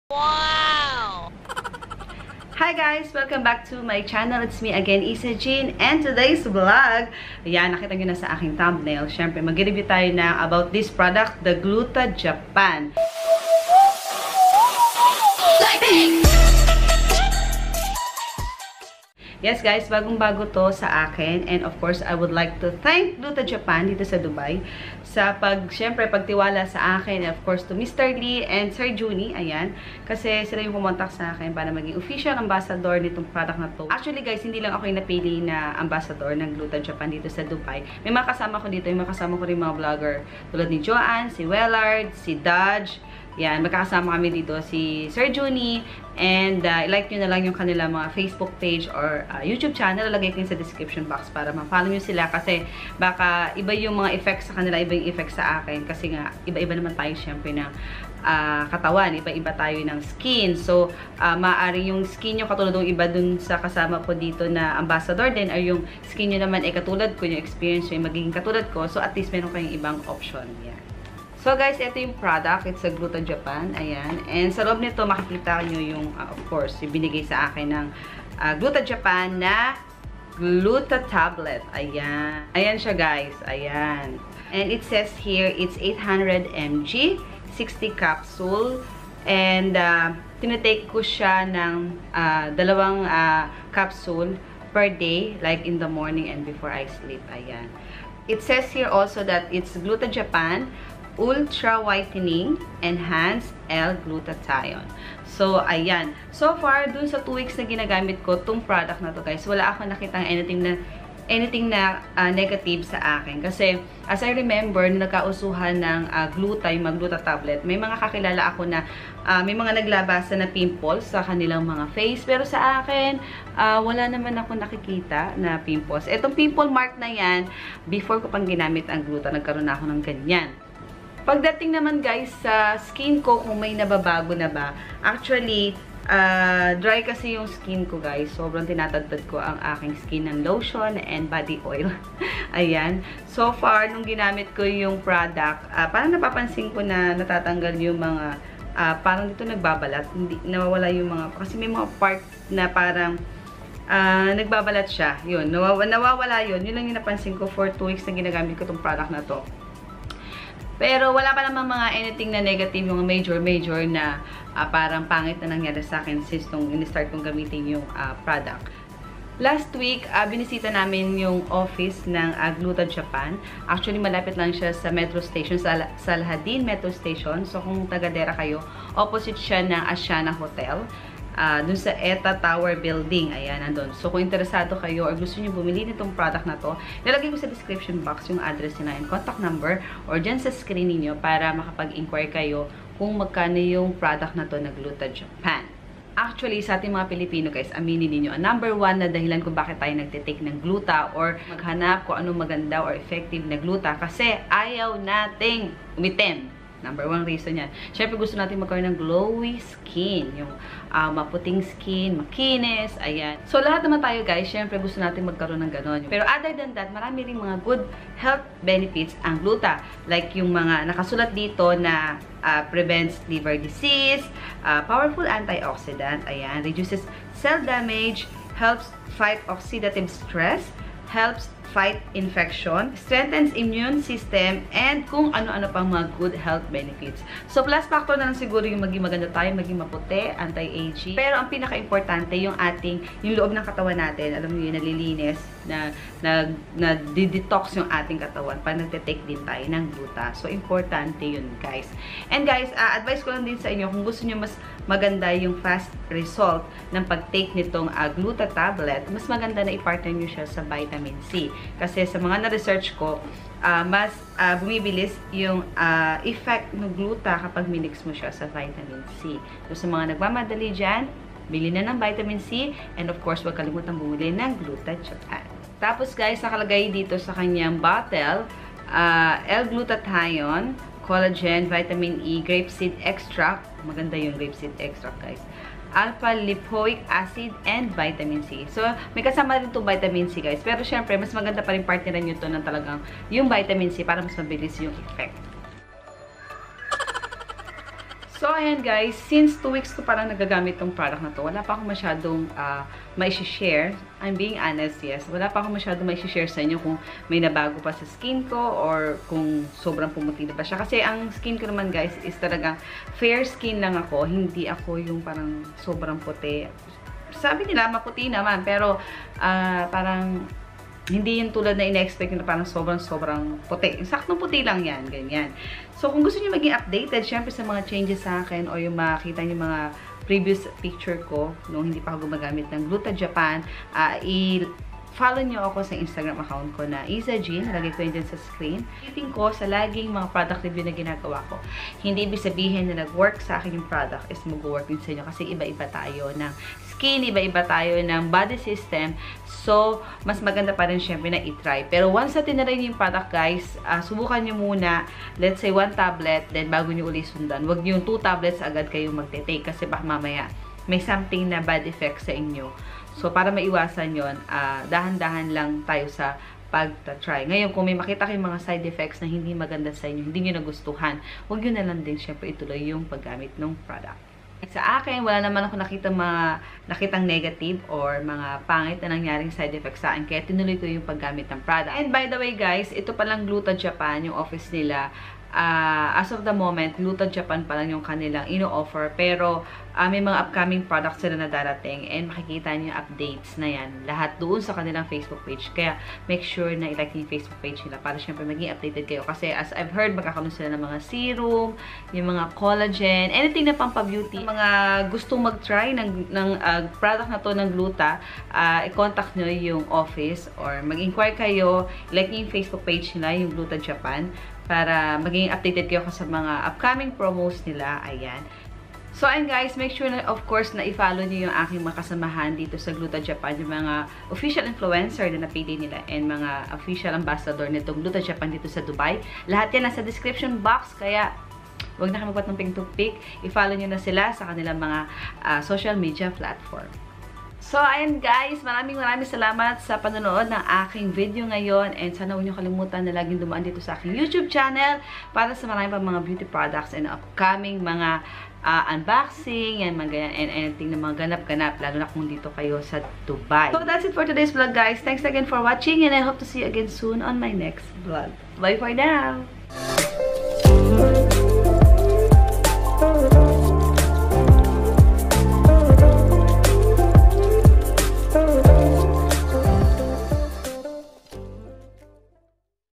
Gluta Japan in Dubai | Product Review
Hey Guys,

Watszup!

Today mag product review tau.. and im very proud na maging isa sa influencer endorser ng product ma toh.if ur interested please keep on watching 😊😁

XoXo
Yza 😚